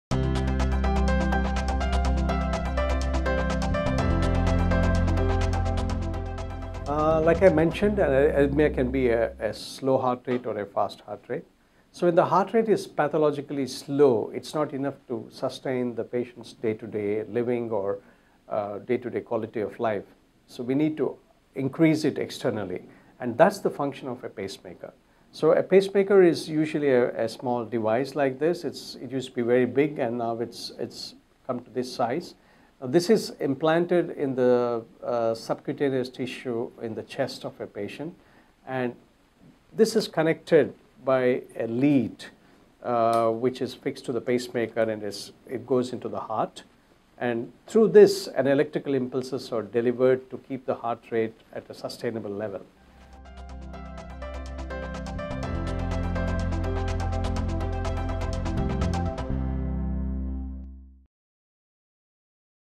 What is a Pacemaker and How Does it Work?
When the heart rate is pathologically slow, it cannot sustain a person's quality of life.  Hence, the need for a pacemaker.  A pacemaker is usually a small device that is implanted in the chest of the patient to deliver electrical impulses and to keep the heart rate at a sustainable level.

To learn more about arrhythmia, join Dr. Ravi Kishore live on Twitter on Wednesday, June 3 @2pm EST